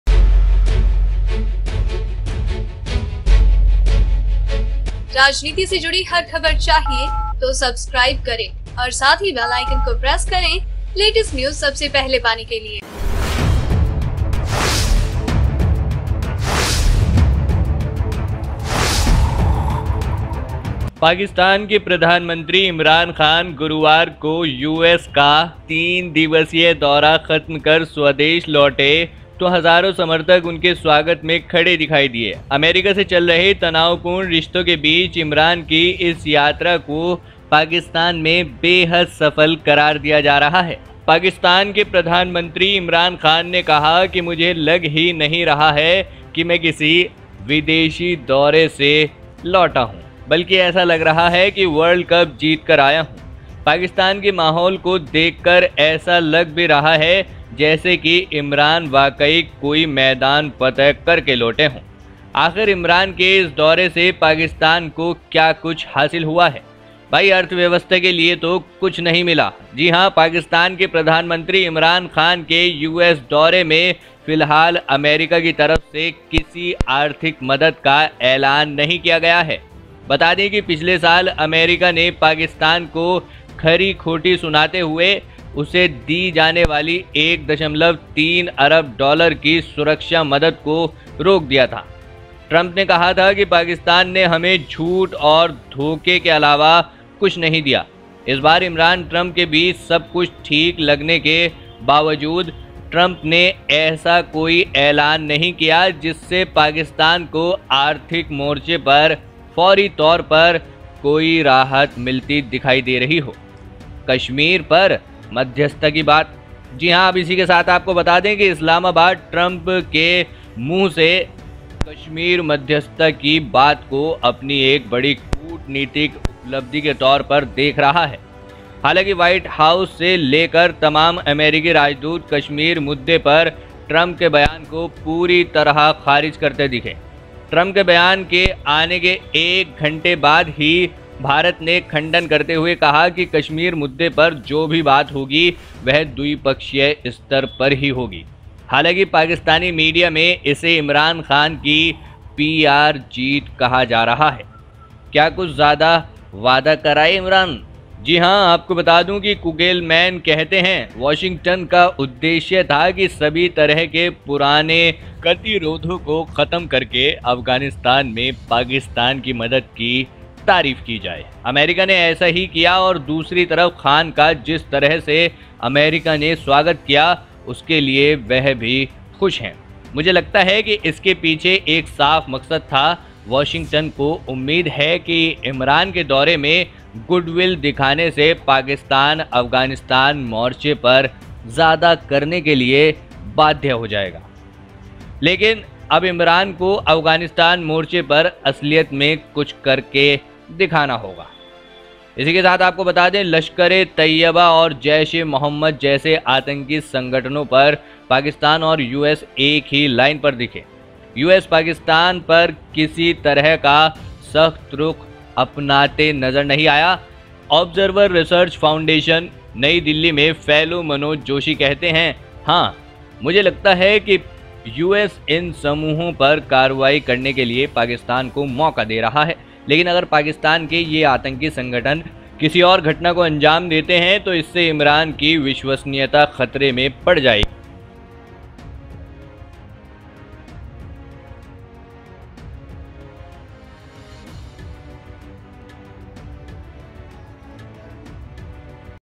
अमेरिका से क्या लेकर पाकिस्तान लौटे पीएम इमरान खान? क्या कुछ ज्यादा वादा कर आए इमरान?
Was Pakistan PM Imran Khan's visit to the US a success? Analysts in Washington, Islamabad and New Delhi assess Pakistani PM Imran Khan's visit to the United States.